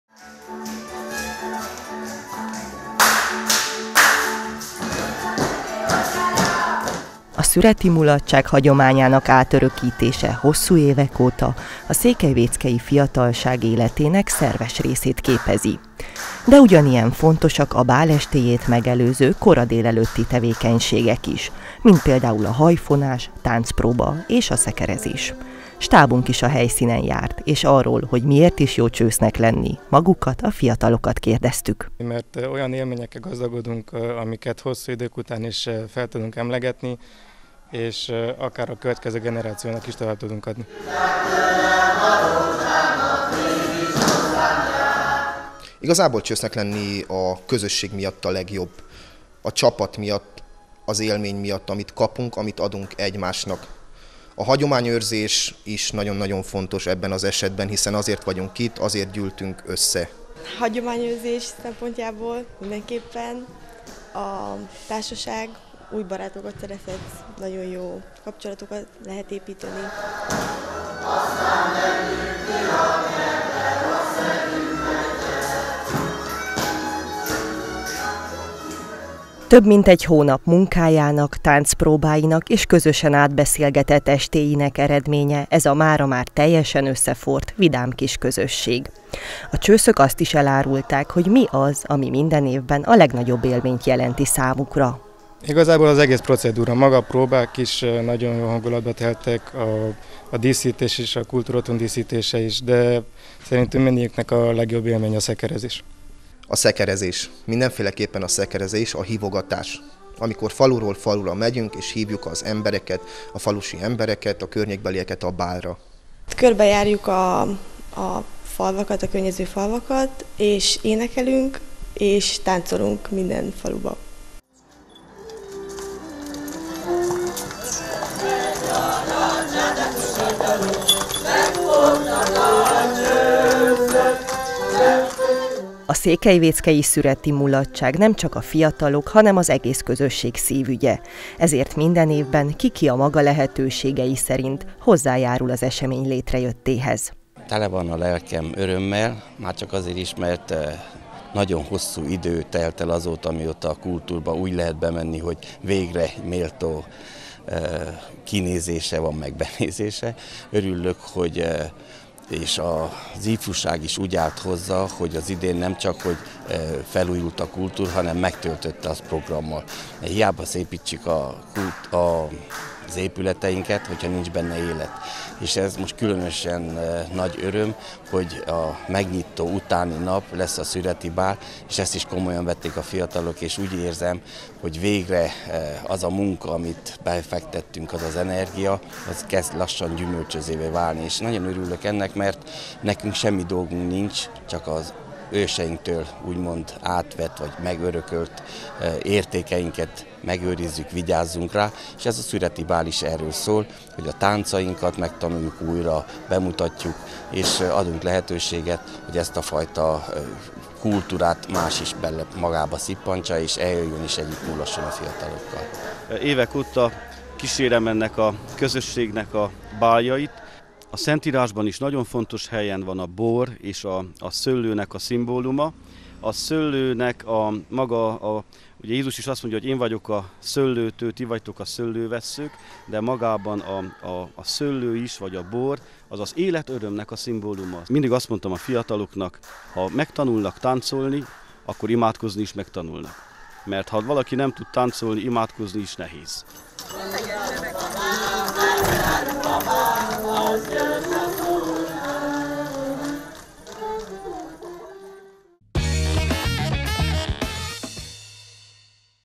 Eljött az ideje a szüreti mulatságnak! – Mozaik
🍇 Látogassunk el Székelyvéckére, ahol éltetve egyik legszebb őszi hagyományunkat, szüretibált rendeztek.

🍷 Nemcsak a csősztánc, a jó zene és a jó bor biztosítja ilyenkor a fergeteges hangulatot, hanem már a bált megelőző készülődés is. Nézzük, miből is áll ez.